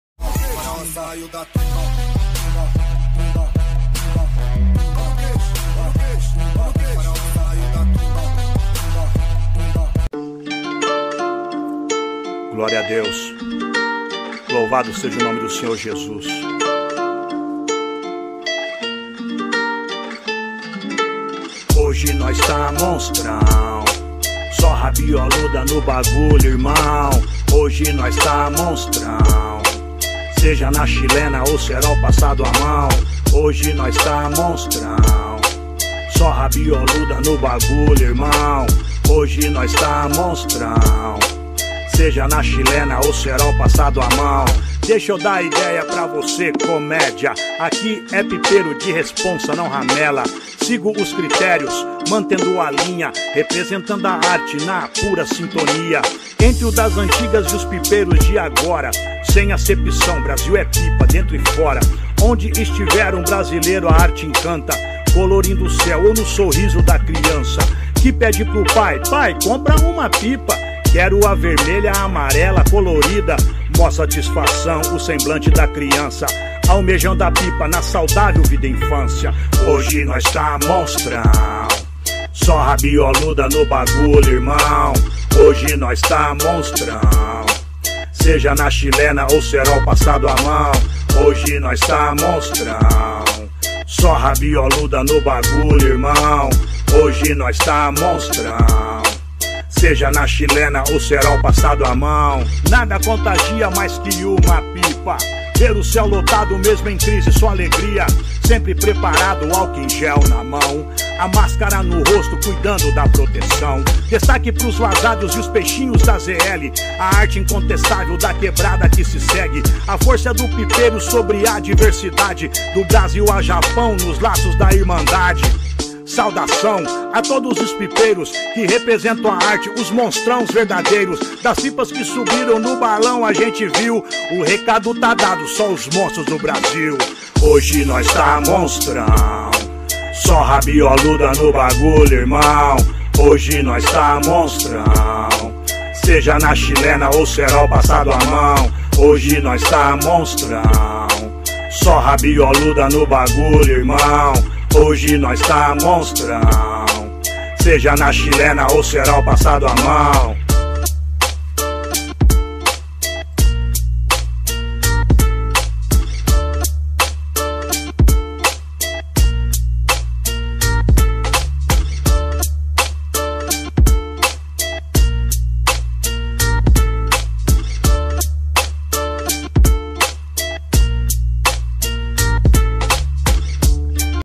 papel simples corte recorte para pipa BATATÃO DE 80 CM# 145
esses papeis são uma encomenda feita pelo meu amigo Carlos" CANAL GERAL DO RÉLO", lembrando que não estou aceitando encomendas de papel nem simples e.nem os cortes em 3D.